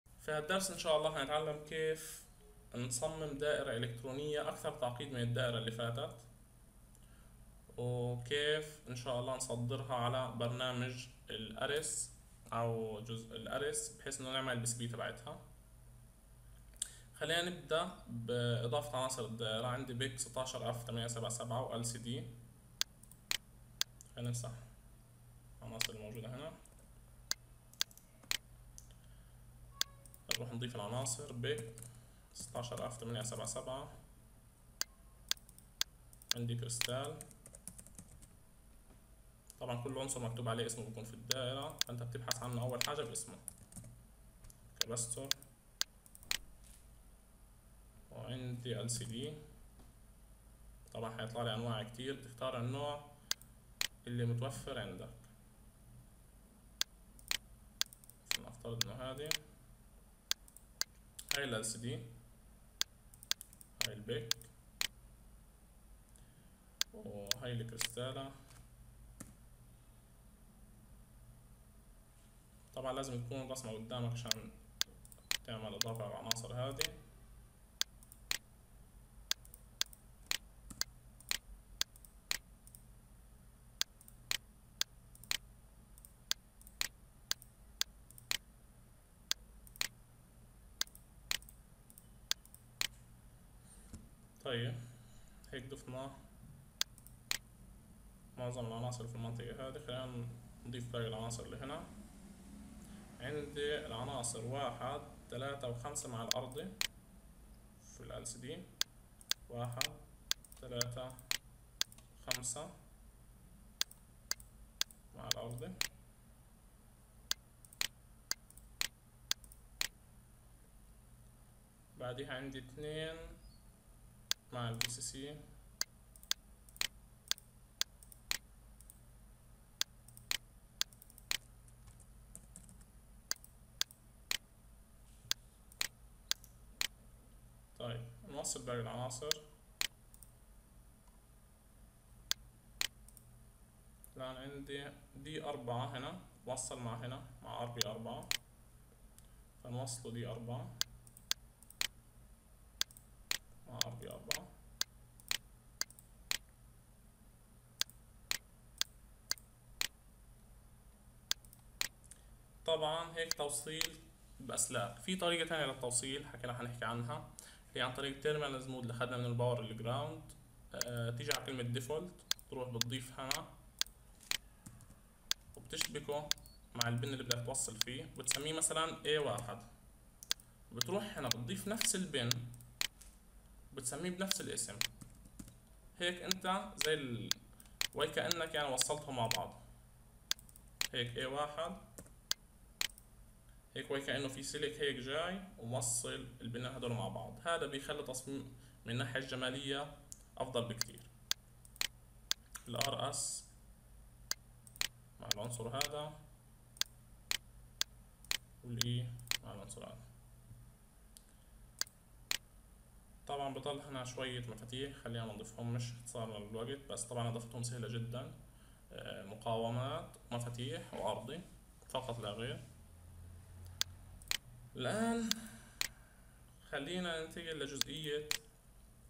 الدرس السادس رسم وتصميم دائرة الكترونية معقدة
• والآن دعونا نفعل دائرة أكثر تعقيدا مع العنصر الذي لا يخرج في المكتبة: